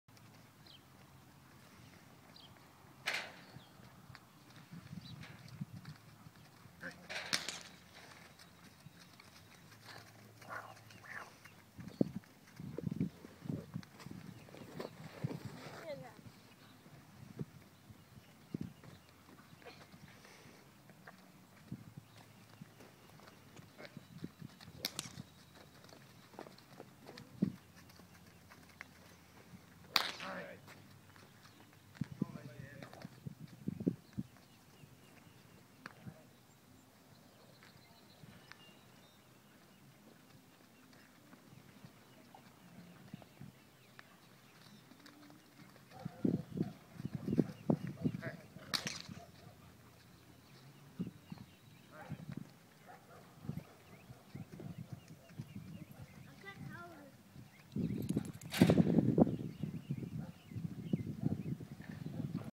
Ranger being lunged in outdoor arena
4 yr old quarter gelding (for sale) being worked out